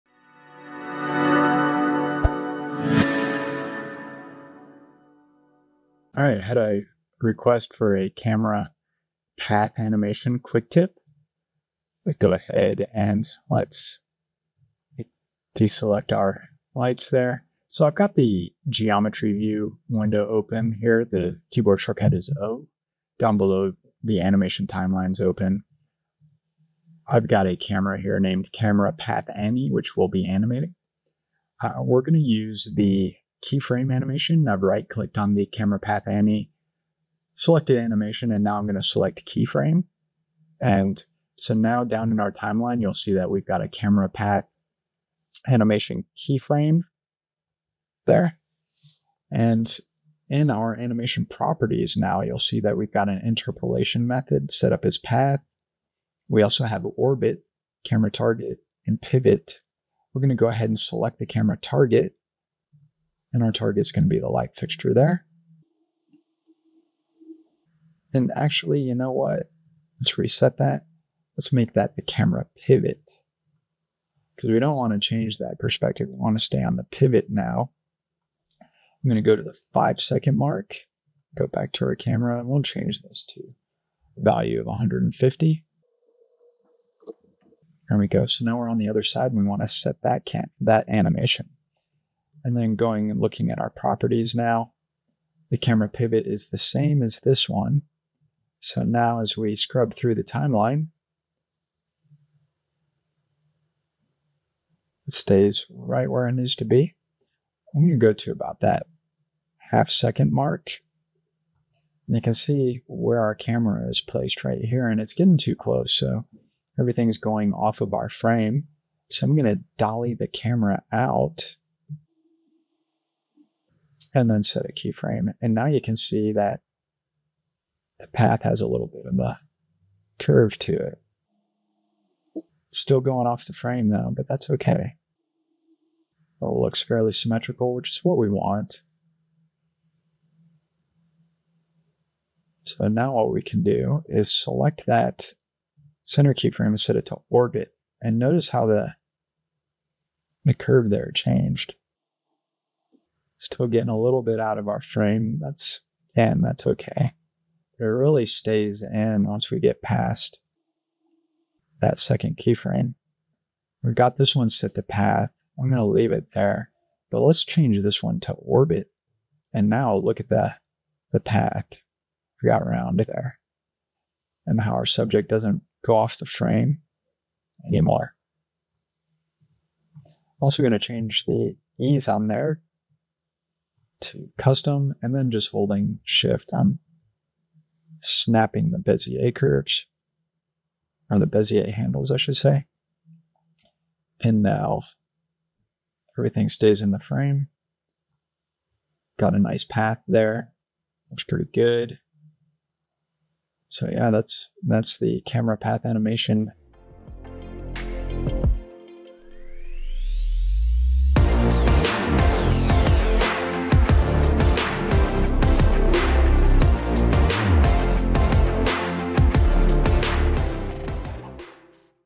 KeyShot Quick Tip - Camera Path Animation
In this KeyShot Quick Tip, we'll take a look at camera path animation, and learn how to create a simple yet dynamic visual.